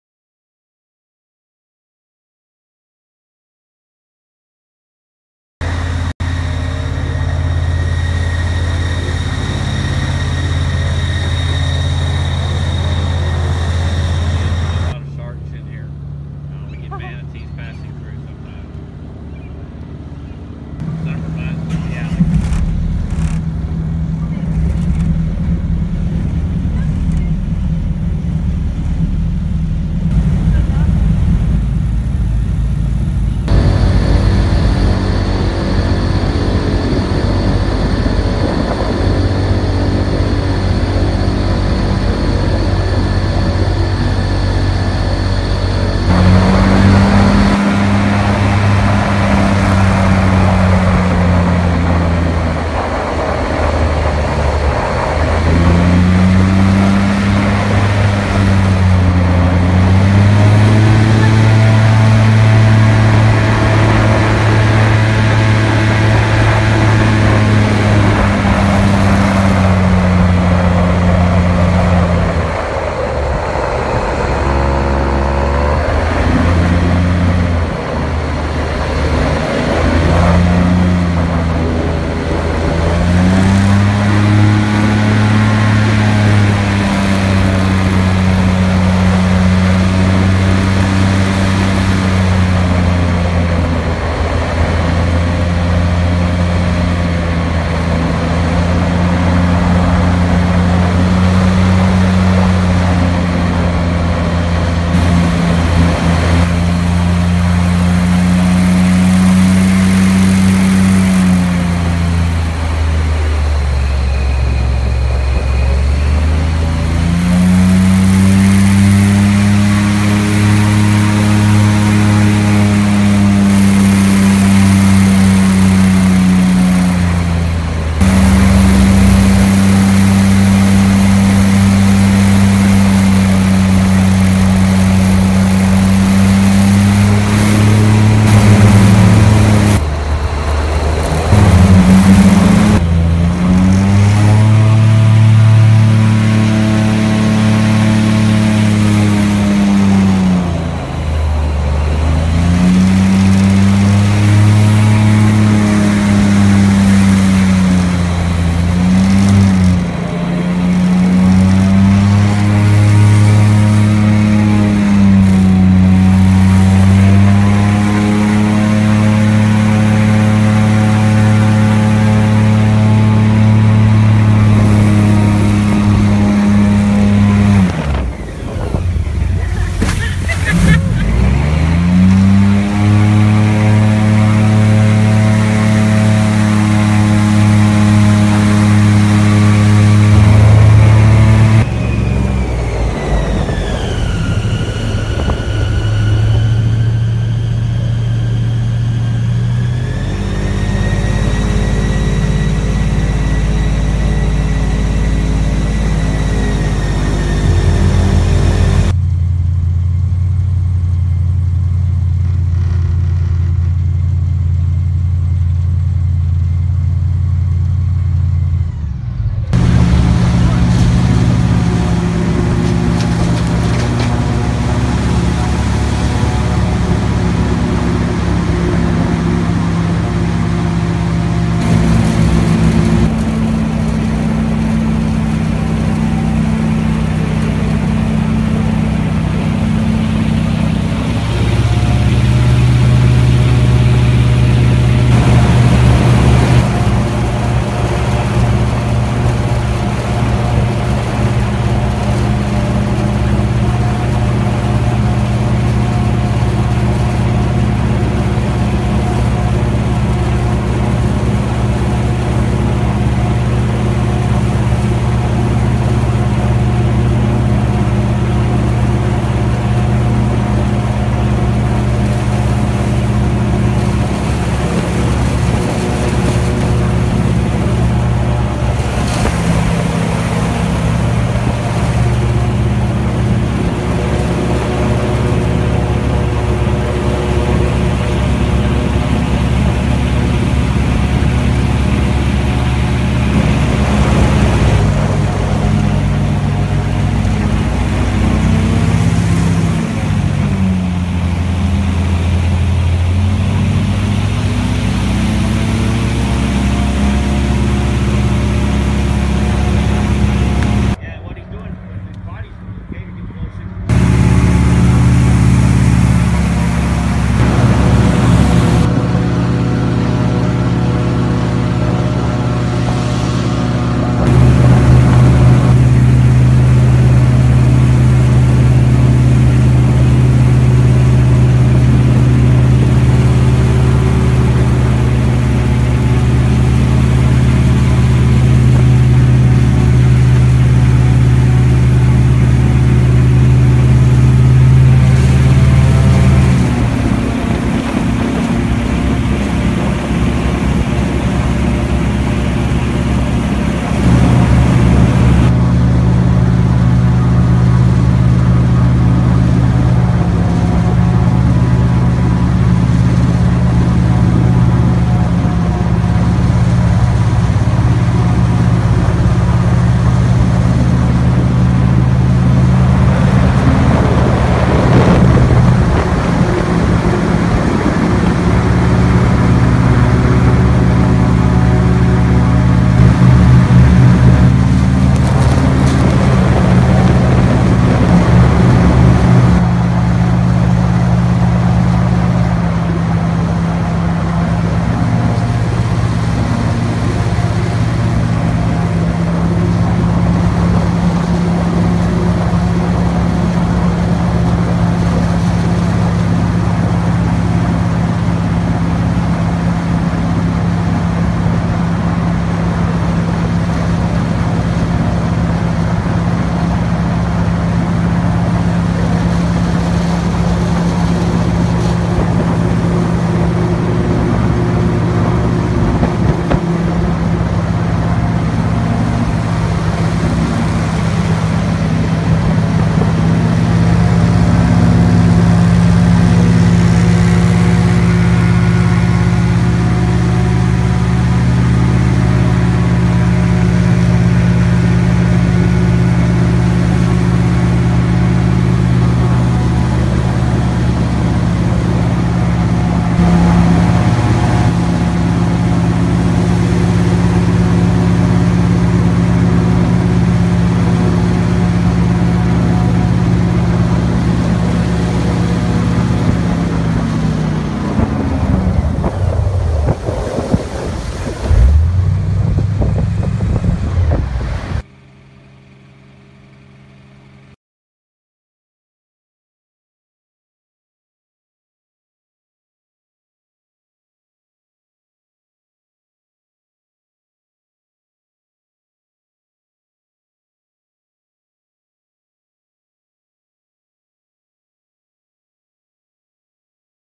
Balade en airboat Everglades
Une belle découverte de la mangrove, quelques alligators, dauphins, raton-laveurs et autres hérons agrémentent la balade
Sans compter les circonvolutions facétieuses de Captain Jack !!